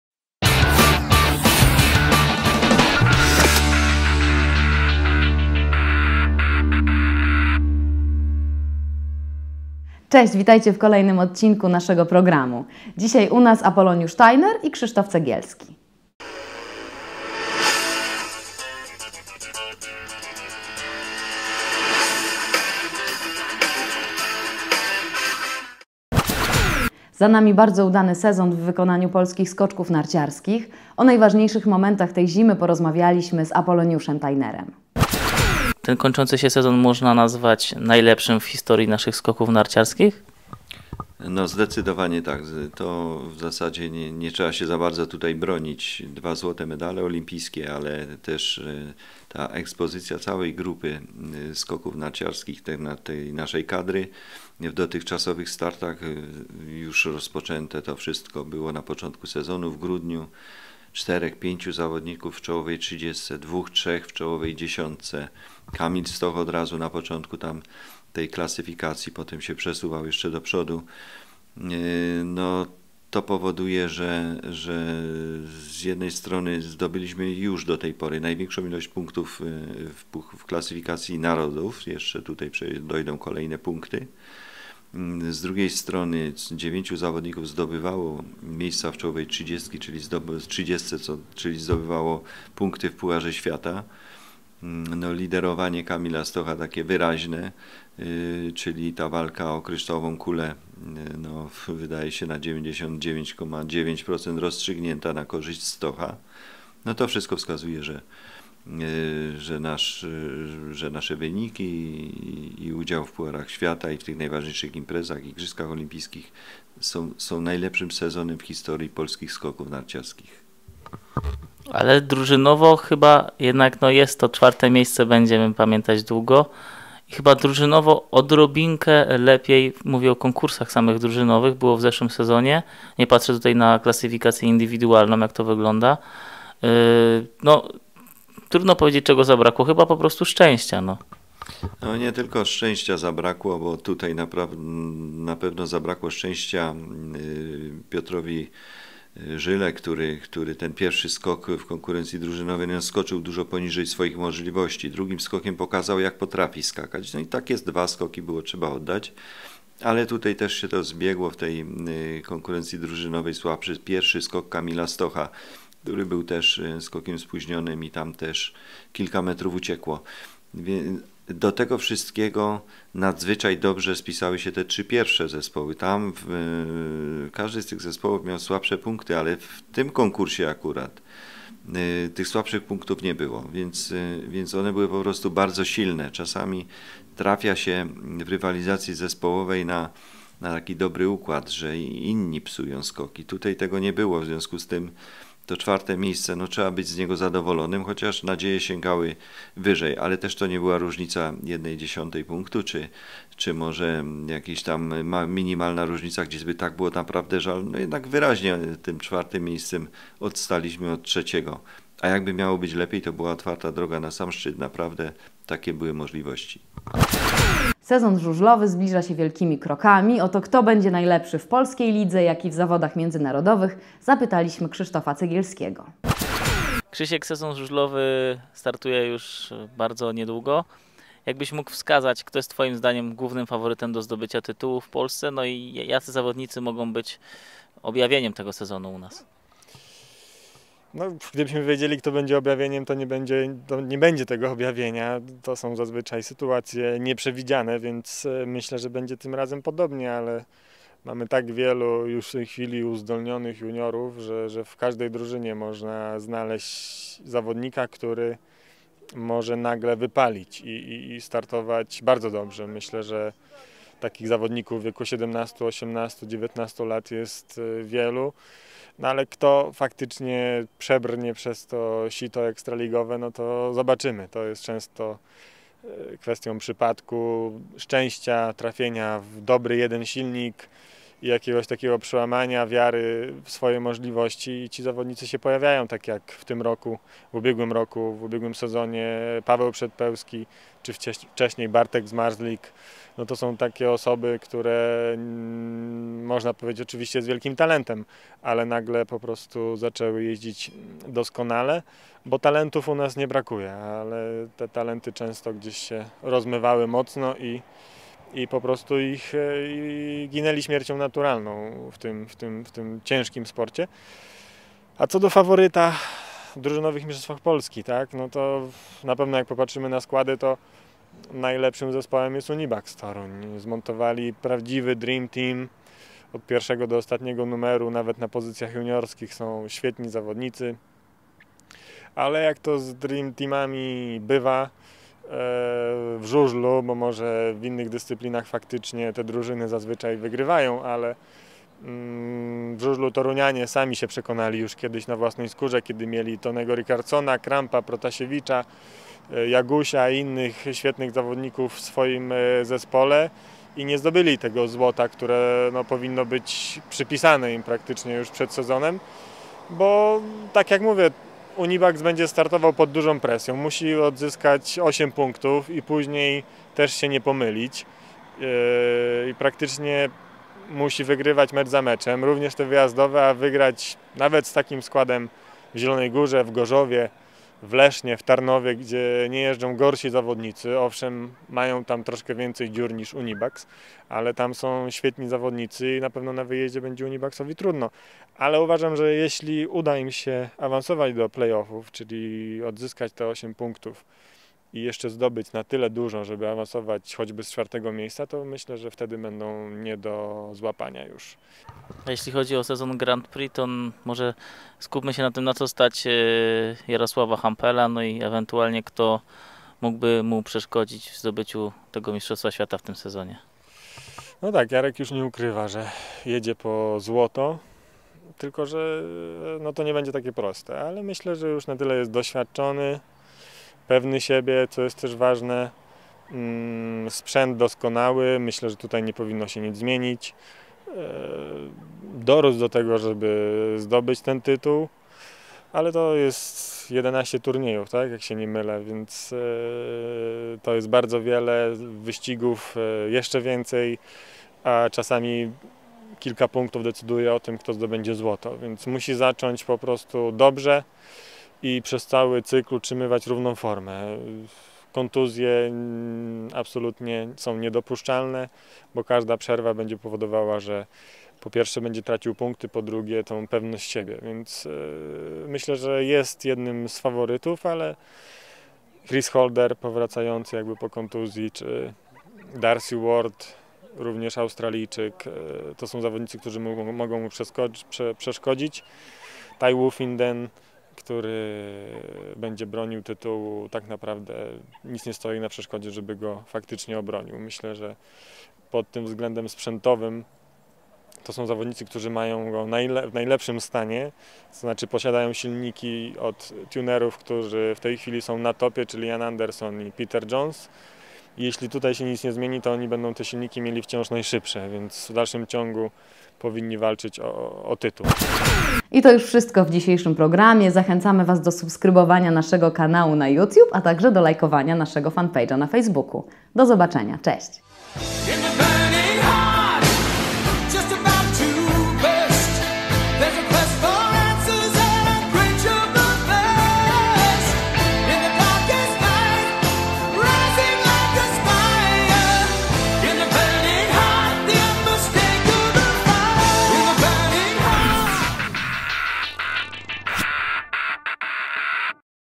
Tajner i Cegielski - Betshow 63. - [ Betsson Polska ]
Apoloniusz Tajner podsumowywuje najlepszy sezon Polski w skokach, a Krzysztof Cegielski przewiduje jak potoczy się Ekstraliga i Grand Prix na żużlu. Prowadzi Anna Kukawska.